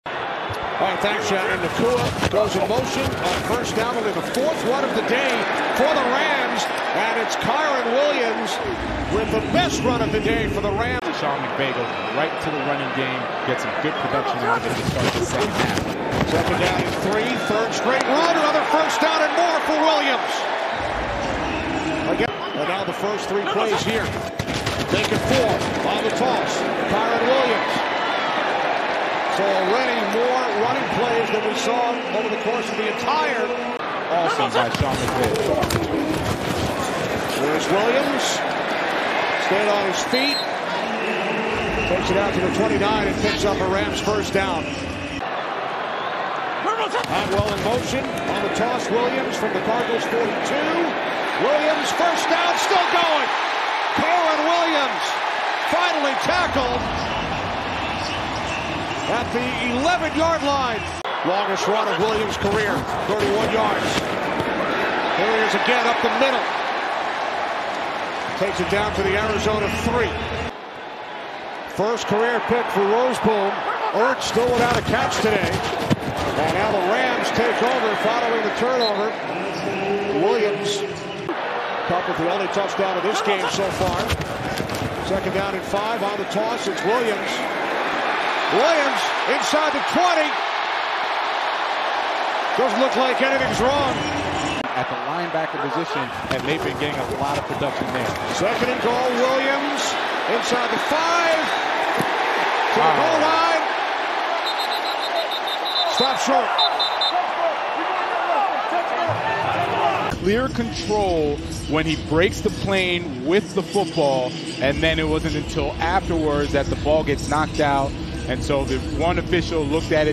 Highlights: Kyren Williams' best plays from 158-yard game vs. Arizona Cardinals in Week 6
Watch all of Los Angeles Rams running back Kyren Williams' best runs from his 158-yard game in the Rams' NFC West showdown against the Arizona Cardinals at SoFi Stadium in Week 6 of the 2023 NFL regular season.